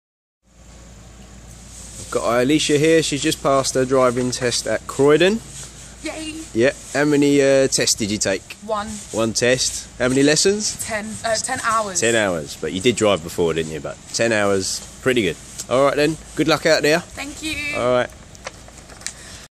1st time pass for Eilisha at Croydon Test Centre.
Well done Eilisha for passing your driving test 1st time at Croydon Test Centre!